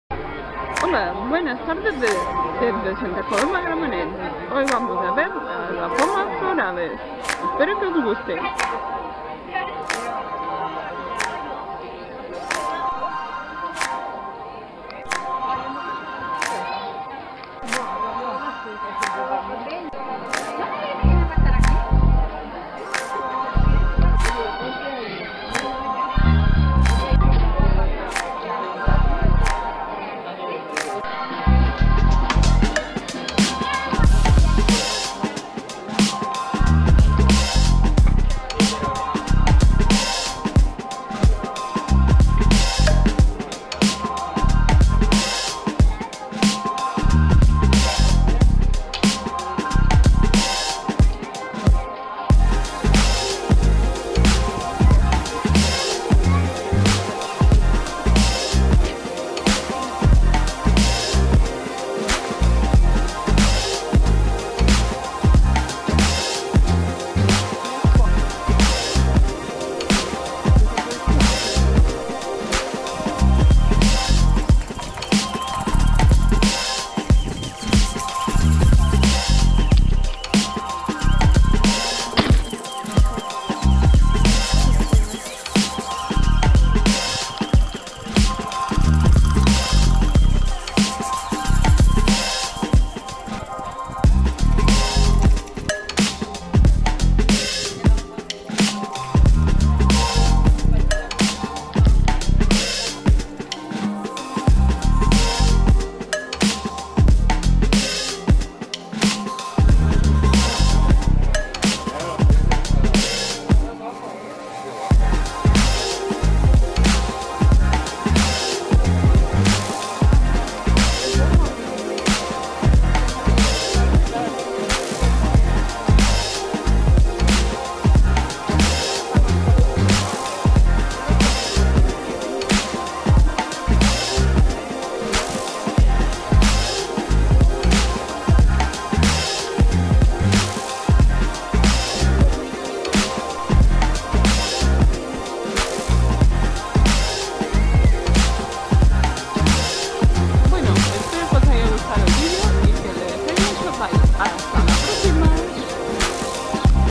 Alfombras florales
Aqui  os traigo un  vlog  de  alfombras  florales  que  hicieron  en  mi  ciudad  .  Espero  que  os guste   y que  le dejeis   muchos  likes    . Buen  finde  a  todos   el  lunes  nuevas  locuras  en  este canal  un  abrazo